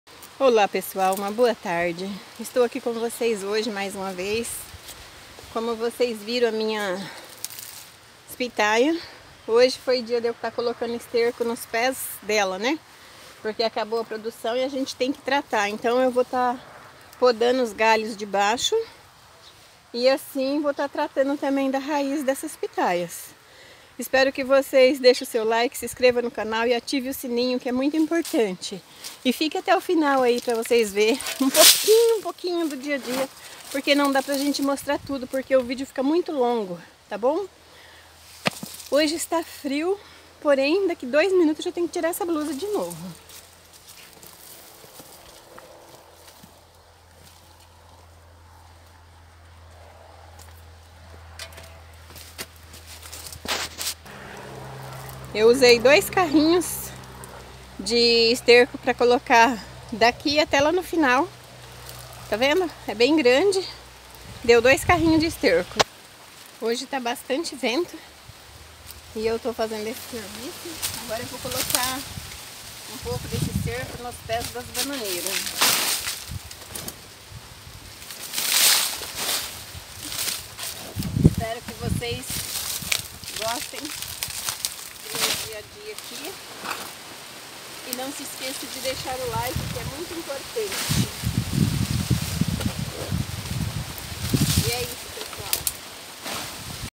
PITAYA VERMELHA!!! CUIDADOS !!!
Depois dos frutos agora só cuidar para a próximo ano.